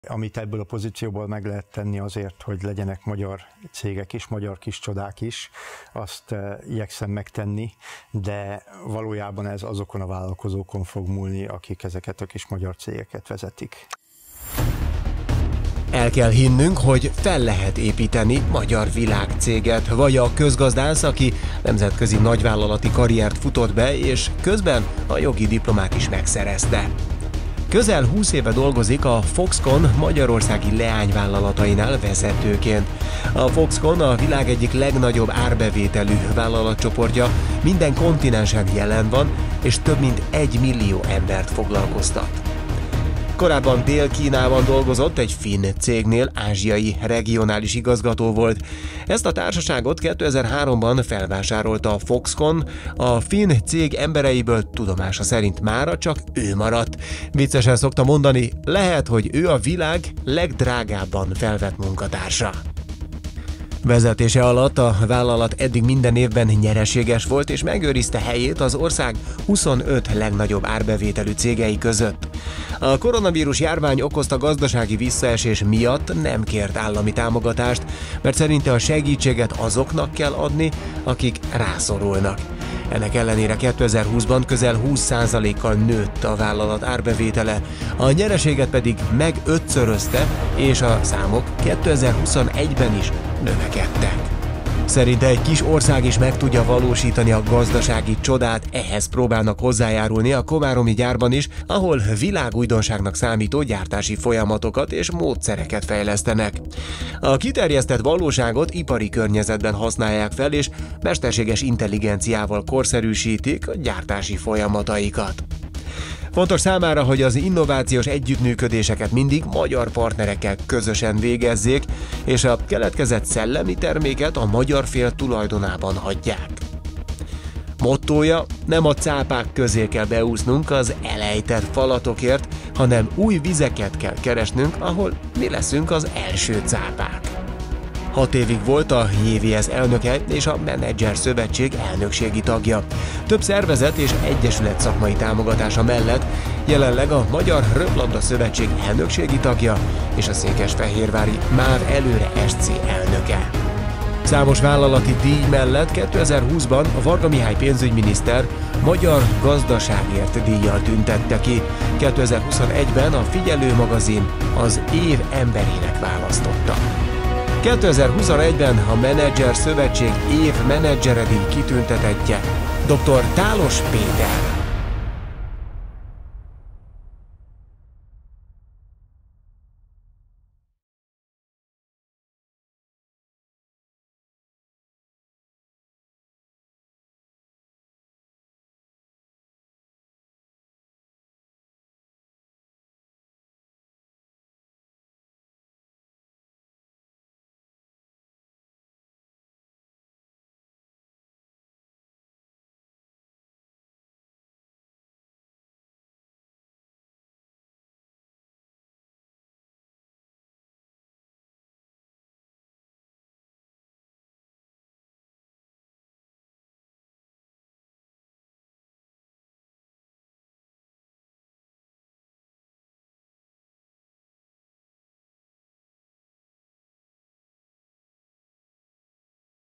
2021 Év Menedzsere Díj kitüntetettje: dr. Tálos Péter
2021-ben a Menedzserszövetség Év Menedzsere Díj kitüntetettje: dr. Tálos Péter a Foxconn csoport alelnöke, a Cloud Network Technology Kft. ügyvezető igazgatója